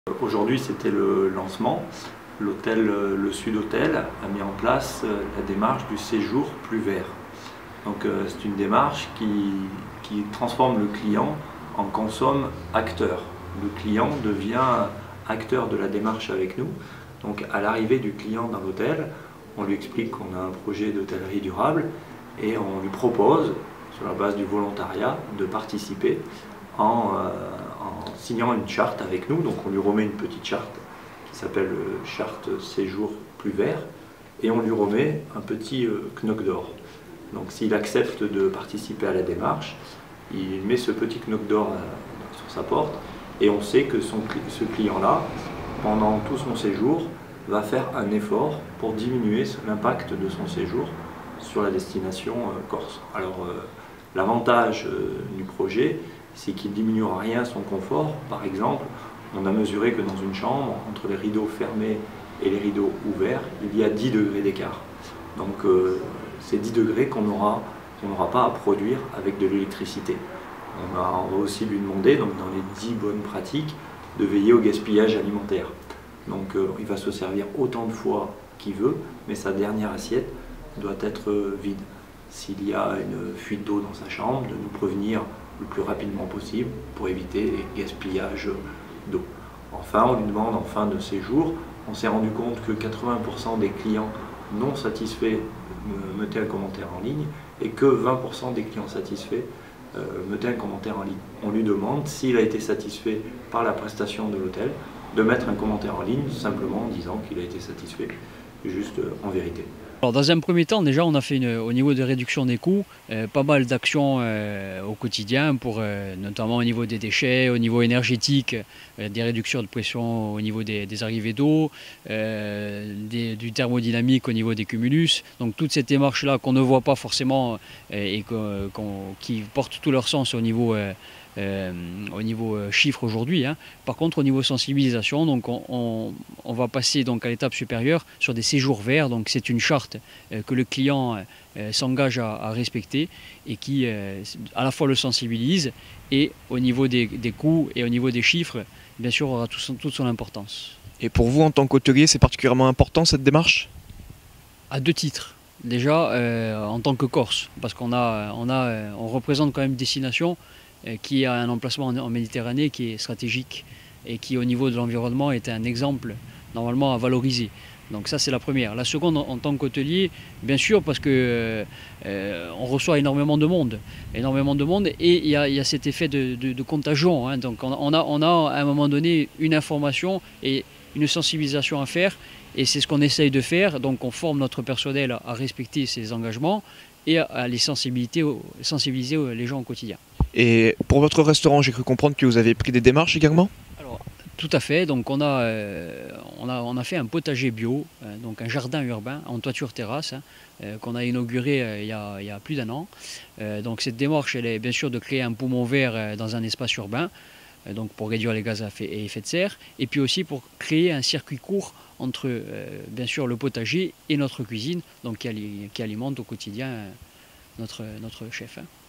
Une charte pour encourager les clients des hôtels à diminuer leur impact écologique
Engager les clients des hôtels à devenir éco responsables, diminuer l’impact environnemental sans entacher la qualité des services proposés, tels sont les objectifs du programme « Rispettu ». Les clients volontaires pourront signer une charte « Hôtellerie durable » et ainsi limiter leur impact écologique lors de leur séjour. Une démarche à laquelle a souscrit le Sud Hôtel à Bastia.

Interviste di Dominique Vaschalde : Duttore cunsultante in osteria durevule.
Intervista di Joseph Bourgeois : Manente di « Sud Hôtel ».